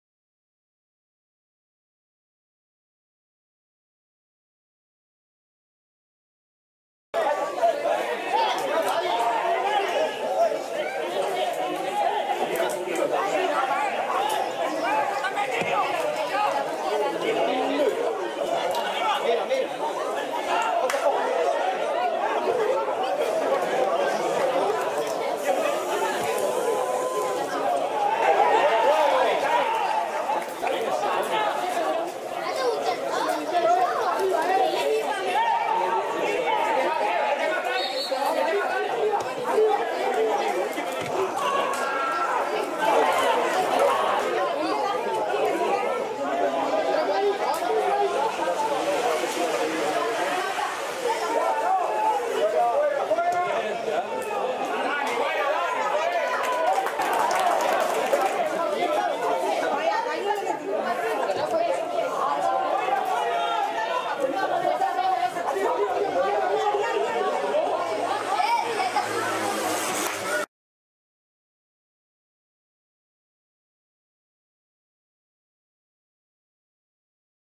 Jugadas de ataque del CD Vera 12 3 2016
Jugadas de ataque del CD Vera. Partido de fútbol UD San Fernando-CD Vera. Tercera División Canaria. Temporada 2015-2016. Jornada 29. 12/3/2016. Campo de Fútbol Municipal Eleuterio Valerón. El Tablero de Maspalomas. San Bartolomé de Tirajana. Resultado final: 2-0.